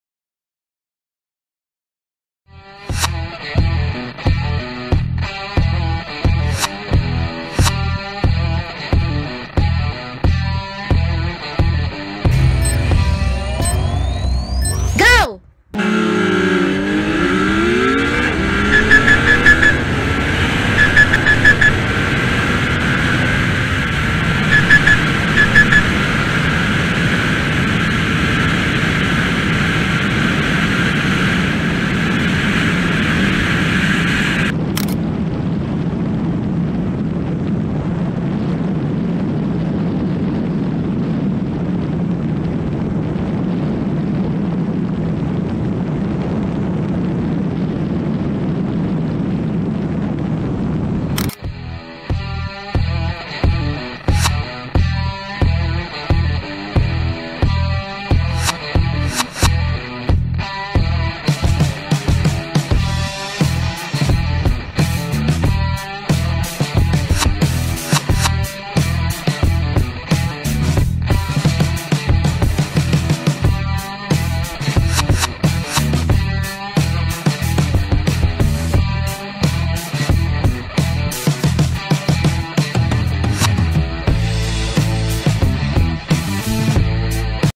Yamaha XSR900 🆚️ Kawasaki Z900 | 0-200kph Acceleration | Top Speed Attempt 🔥
CTTO

🛑 XSR900

🛑 Z900